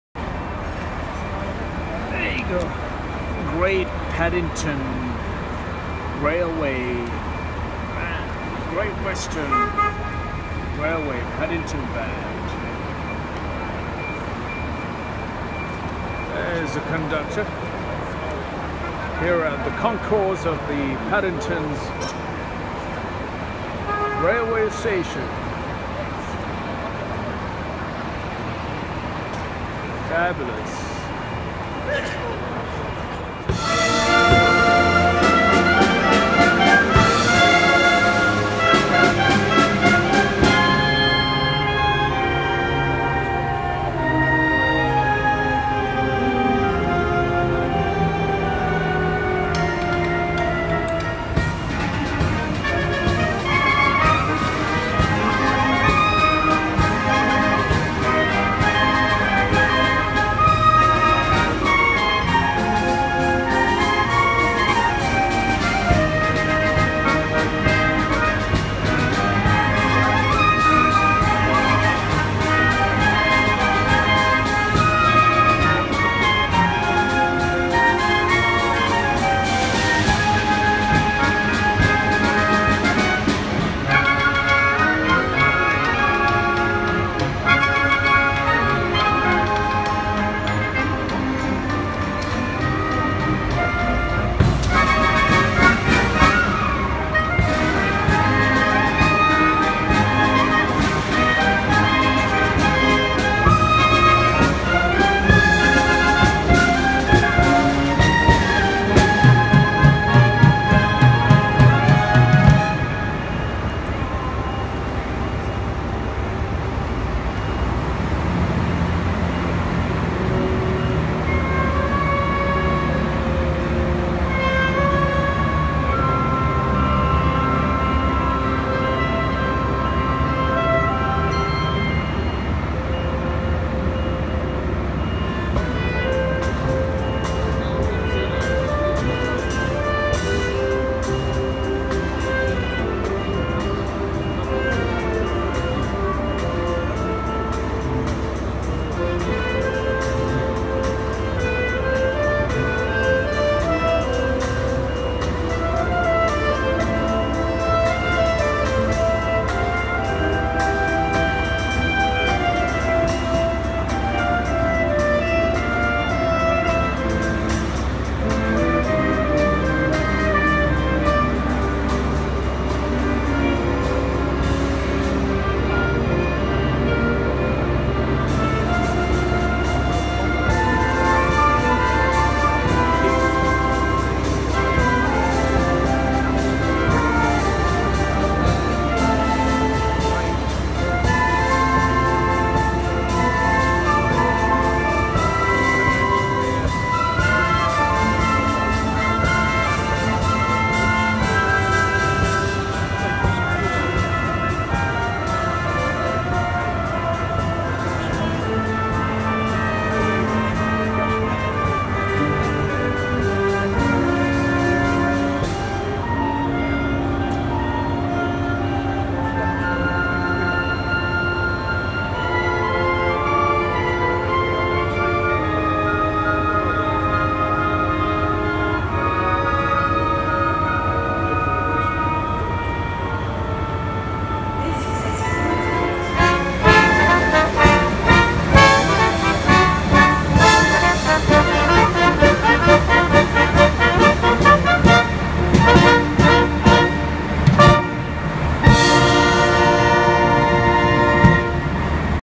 Great-Western-Paddington-Railway-Band
English: The Great Western (Paddington) Railway Band started out as a brass band for employees of the Great Western Railway in the UK.

Now it is open to musicians from all walks of life, and in addition to other venues, plays regularly in the concourse of Paddington Railway Station in London, entertaining the public and raising money to help the band to continue.